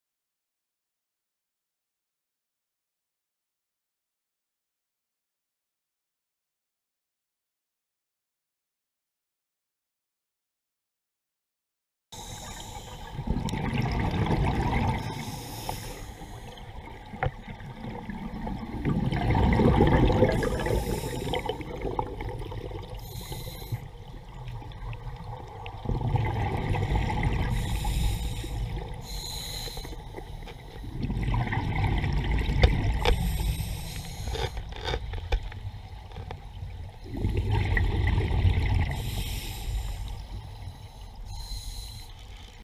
Morena primer plano
Un primer plano de una morena.